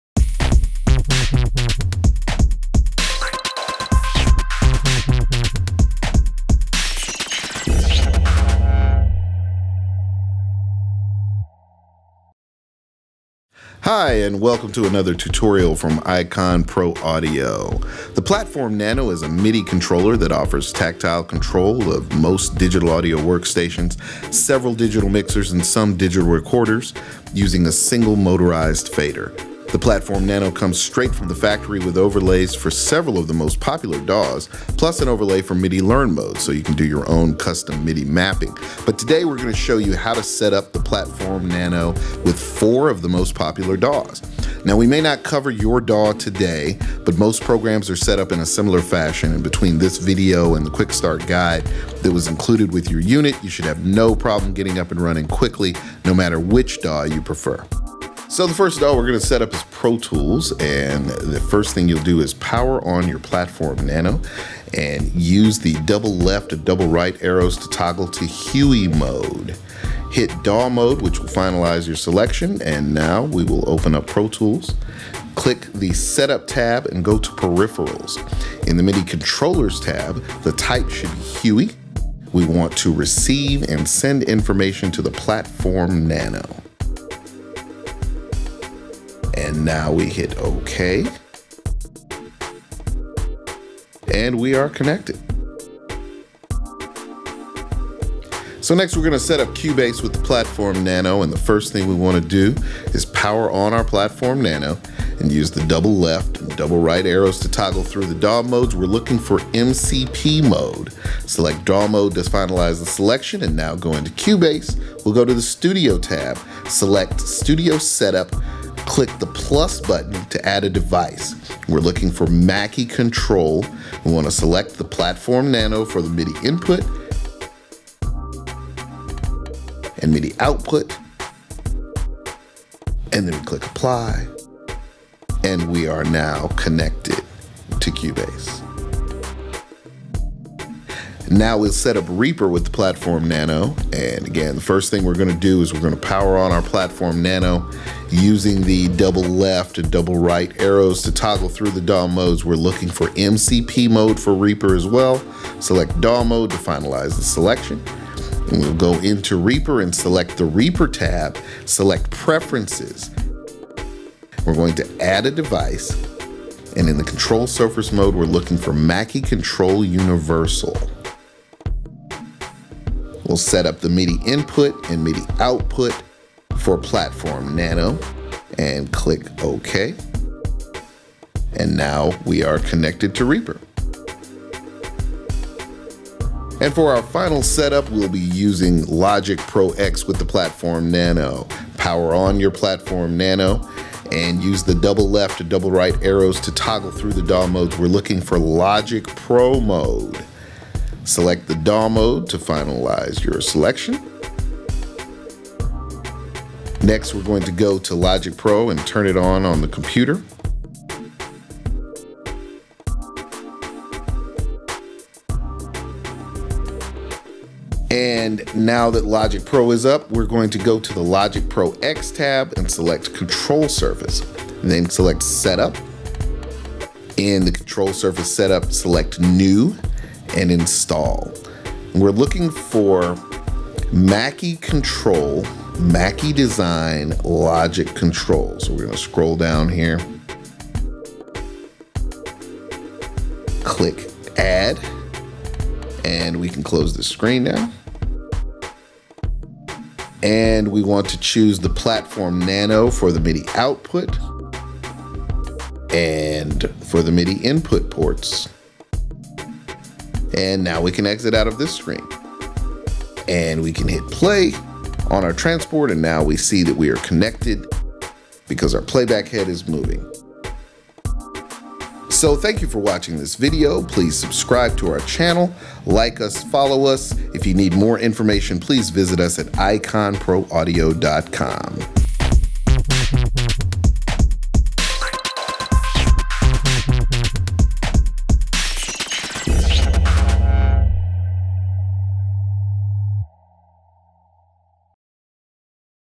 How to Setup PlatformNano With Your DAW
The PlatformNano is a MIDI controller that offers tactile control of most digital audio workstations, several digital mixers and some digital recorders, using a single motorized fader.

The PlatformNano comes straight from the factory with overlays for several of the most popular DAWs plus one for MIDI learn mode so you can do your own custom midi mapping.

Today we're going to show you how to setup the PlatformNano with four of the most popular DAWs.

We may not cover your DAW today, but most programs are setup in a similar fashion.

Between this video and the quickstart guide that was included with your PlatformNano, you should have no problem getting up and running quickly no matter which DAW you prefer.”

For our first setup, we will be using Pro Tools .

The first thing we want to do is power on our Platform Nano Use the double left and double right arrows to go to HUI mode. Hit DAW mode to finalize our selection.

Next you want to open up our DAW.

Click the setup Tab. Go To Peripherals.

In the MIDI Controllers Tab, the type should be HUI.

We want receive and send information to the Platform Nano. Hit ok. Now we are connected.

Next ,we are going to setup Cubase with the Platform Nano. The first thing we want to do is power on our Platform Nano.

Use the double left and double right arrows to toggle through the
DAW modes. We are looking for MCP.

Select DAW mode to finalize the selection.

We are going to go into Cubase.

Go to the Studio Tab. Select studio setup.
click the plus button add a device. We are looking for Mackie
Control. We want to select the Platform Nano for the MIDI input and output. Click Apply and we are now connected.

Next we are going to setup Reaper with the Platform Nano. The first thing we want to do is power on our Platform Nano.

Use the double left and double right arrows to toggle through the DAW modes. We are looking for MCP.

Now we are going to go into Reaper.

Select the Reaper tab and select preferences.

We are going to add a device. In the control surface mode we are looking for Mackie Control Universal.

The MIDI input and output should be the Platform Nano. select Ok.

Now we are connected .

For our last setup, we will be using Logic Pro X with the Platform Nano. Power on our Platform Nano.

Use the double left and double right arrows to toggle through the DAW modes. We are looking for Logic Pro Mode.

Our next step is to turn on Logic Pro on the computer .

Now that Logic Pro is up, lets go to the Logic Pro X tab. Select Control surface , then select setup.

In the control surface setup, select new. Install.

We are looking for Mackie Control , Mackie Design, Logic Control.
Add. close this screen.

We want to choose the Platform Nano for the MIDI Output and Input ports.

We can exit out of this screen now. Im going to hit play on my transport and now you will see that we are connected now because
my playback head is moving .

Go to iconproaudio.com for more information as well as firmware updates.